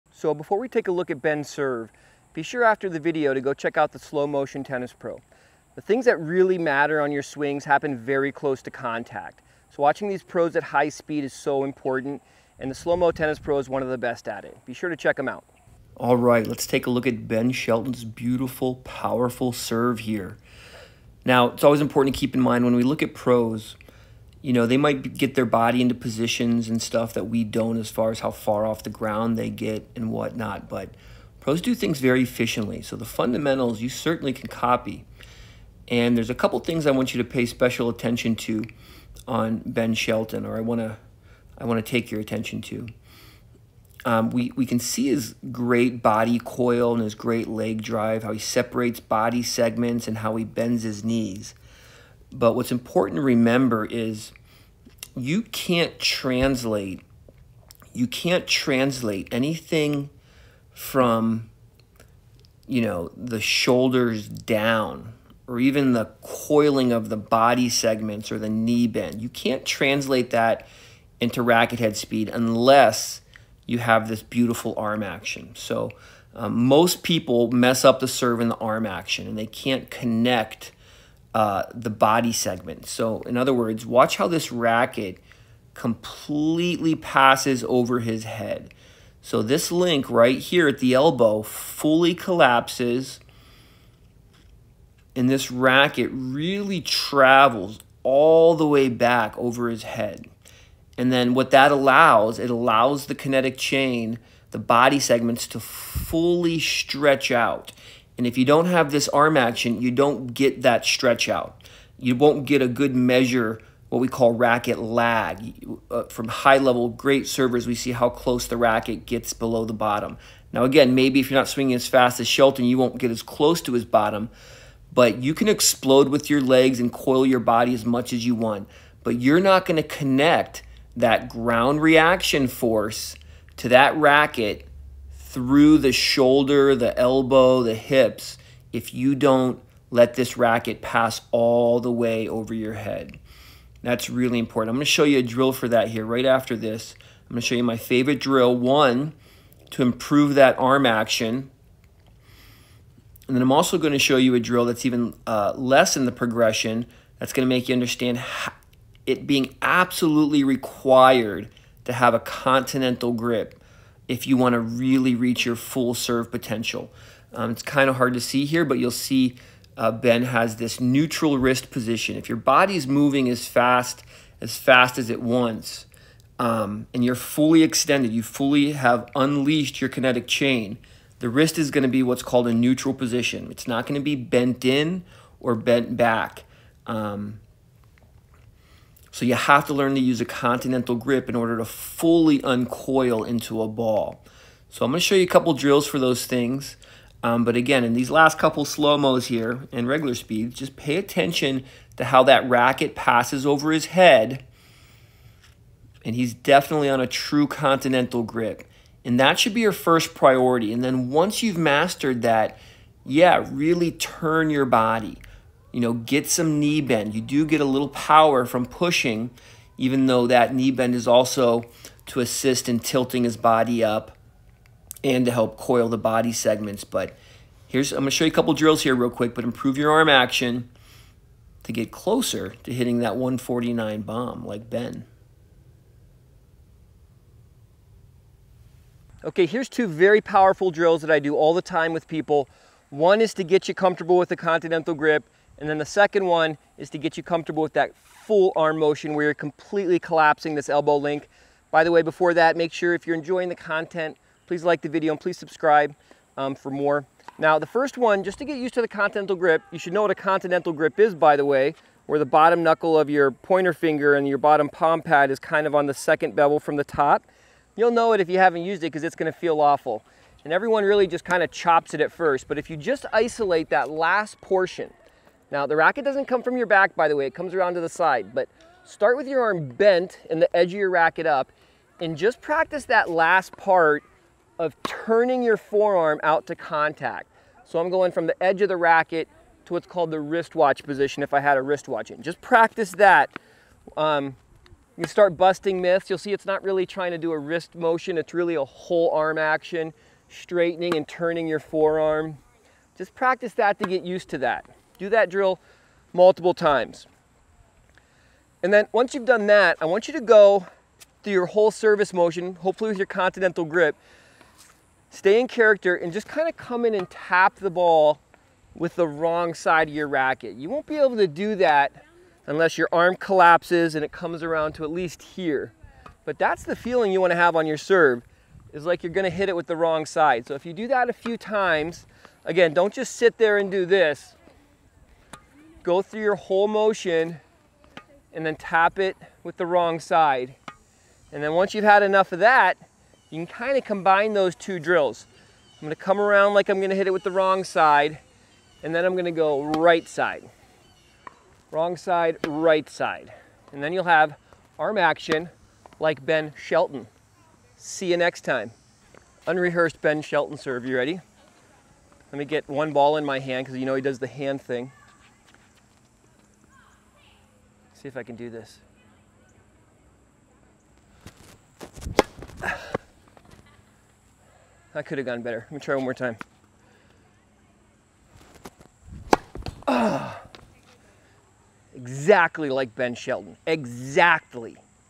Ben Shelton Serve Analysis | How Does He Hit A 149mph Tennis Serve
If you are Ben Shelton hit 149mph serves at the 2023 US Open right now you are watching some of the best serve fundamentals in the game. In this video we go over where most players mess this up and give a couple serve drills that can get you on your way to serving harder.

🎓 Check out my resources to help you improve including free courses